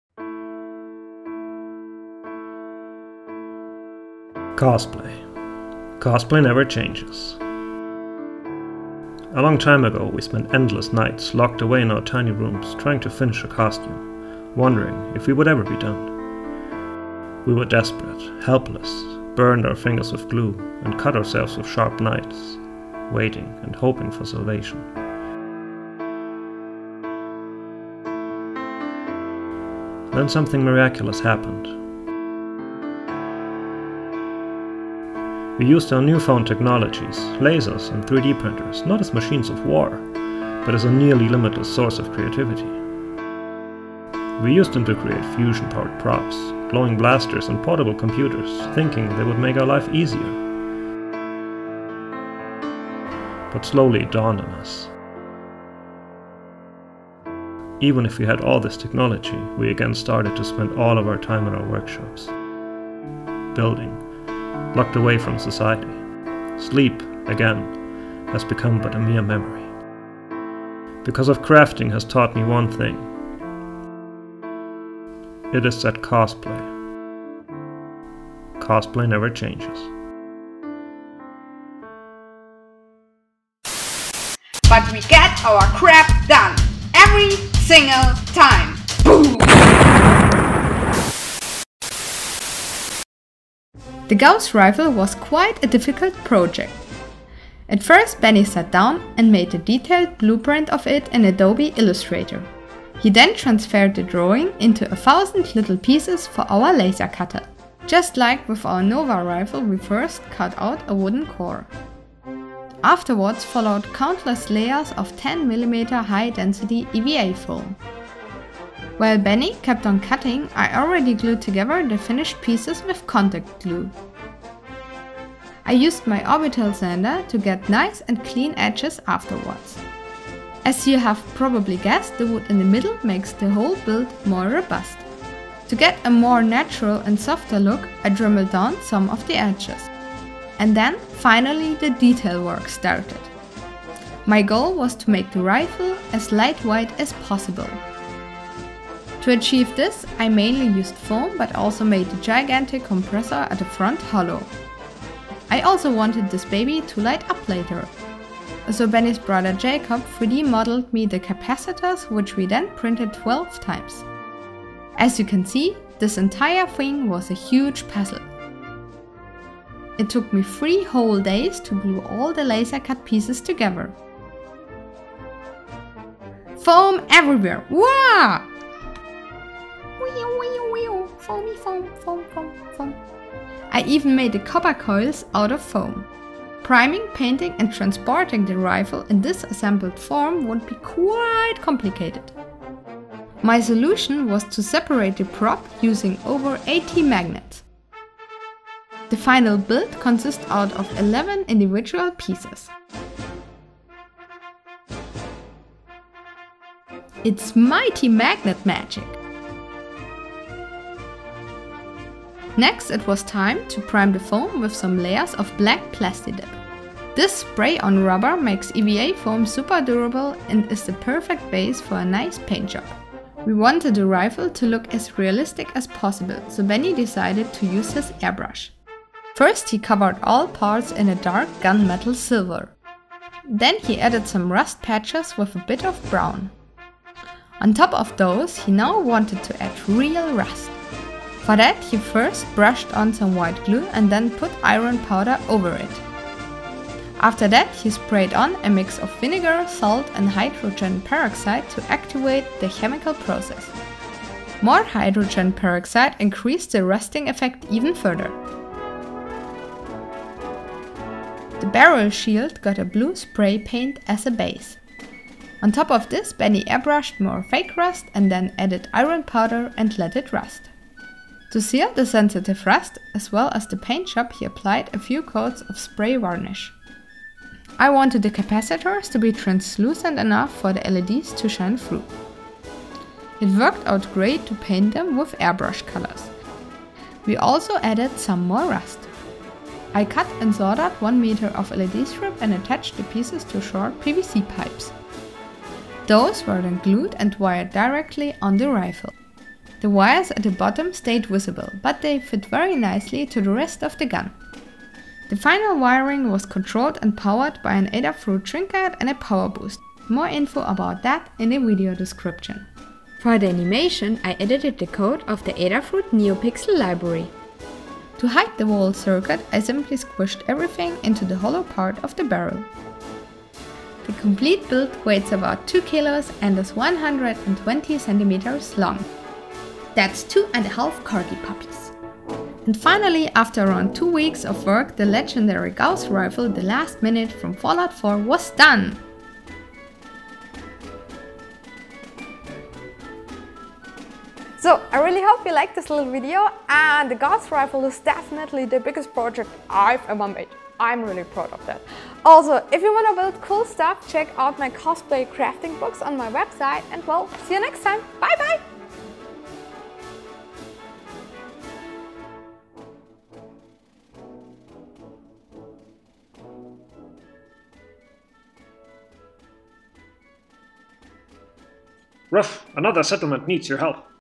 Fallout 4 - Building a Gauss Rifle Replica (The Last Minute)
The Gauss Rifle (The Last Minute) was my favorite weapon in Fallout 4! So it was the perfect project for my upcoming costume! Made mainly out of laser cut EVA foam as well as a few 3D printed parts - it took Benni, his brother Jakob and me two weeks to bring it together.
- Svetlana

For the rusting effect we bought iron powder from eBay and hydrogen peroxide (3%) form the local pharmacy.

Crafting Products:
EVA Foam
Box Cutter
Contact Glue
Dremel Snake

LED Stuff:
LED strips
Soldering Iron
Arduino Uno
Slide switch
Helping Hand

Paint:
Airbrush Set US/CA
Airbrush Set EU
Water Filter
Cleaning Station
Vallejo Model Air Paint
Vallejo Game Air Paint
Airbrush Cleaner
Masking Tape